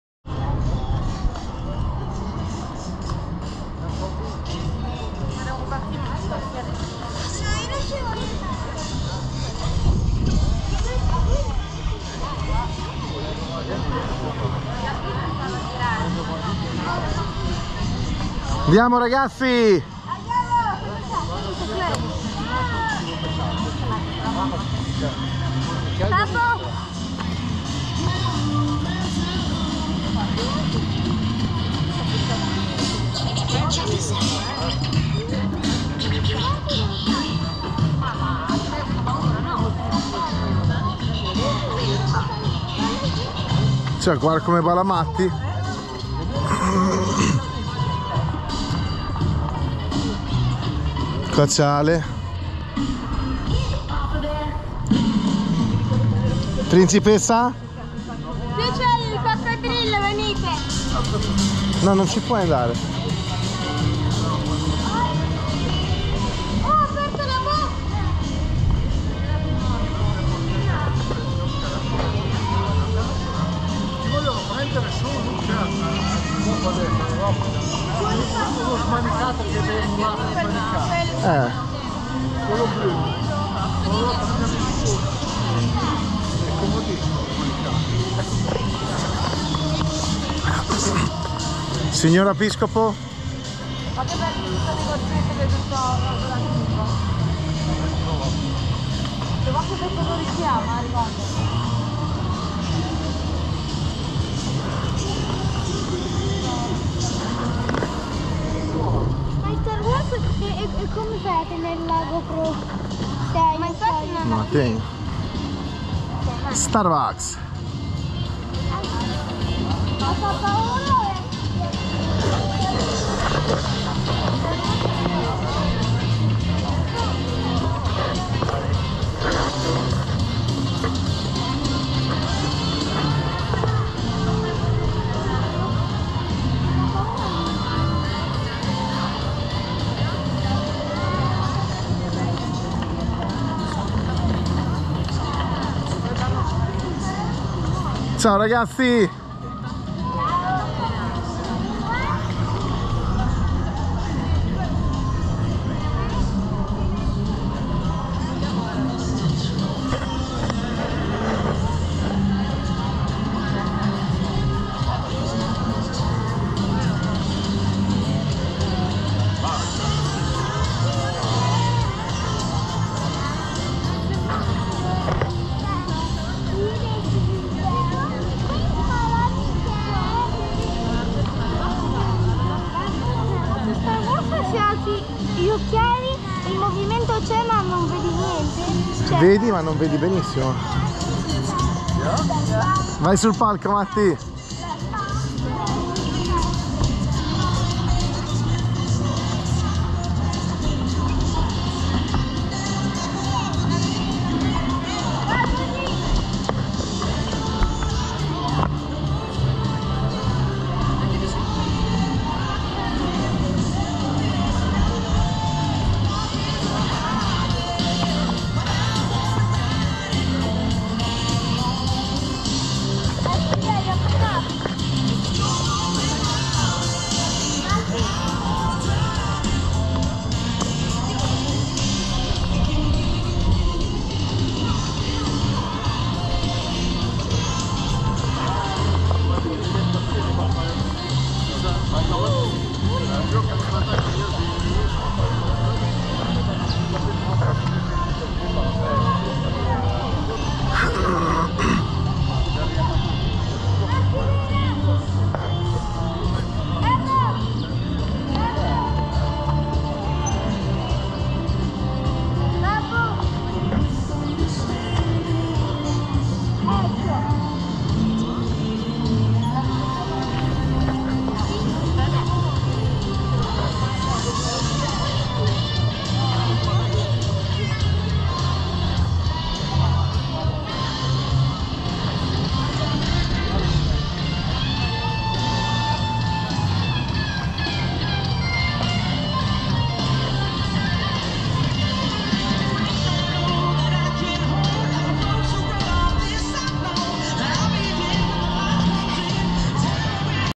EURODISNEY 2022 : DISNEY VILLAGE
Passeggiata alla scoperta del Disney village !!!